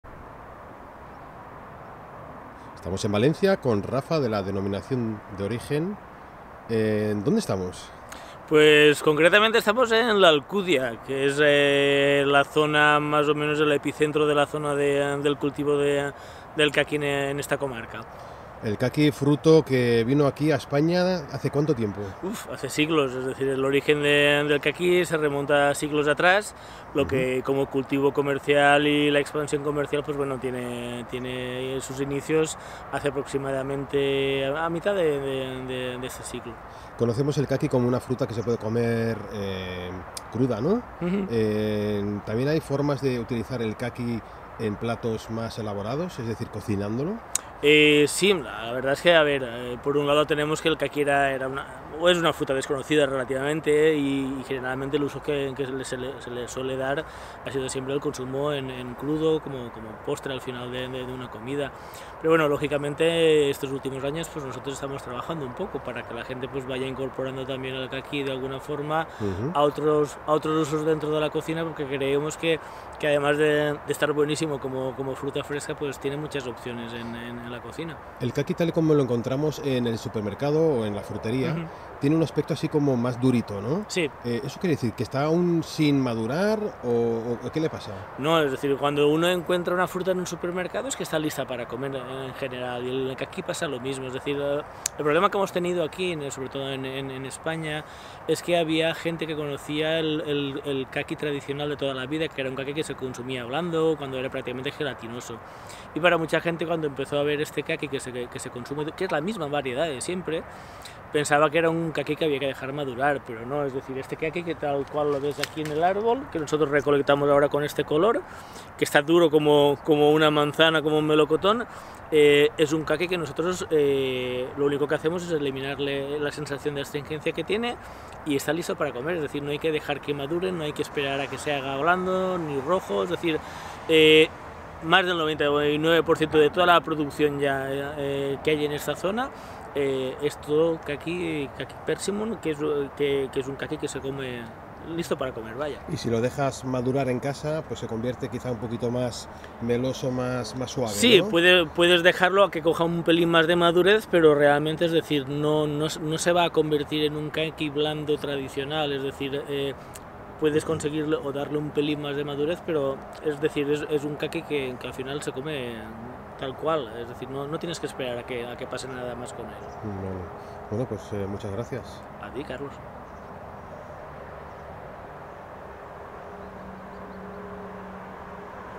Kakis. Todo lo que necesitas saber sobre el kaki o caqui
El responsable de la D.O. Rafa Perucho nos saca de dudas a muchas de las preguntas que tenemos sobre esta fruta. En unos cuantos minutos nos explica con detalle el mundo del kaki.

¡Y déjame tus comentarios!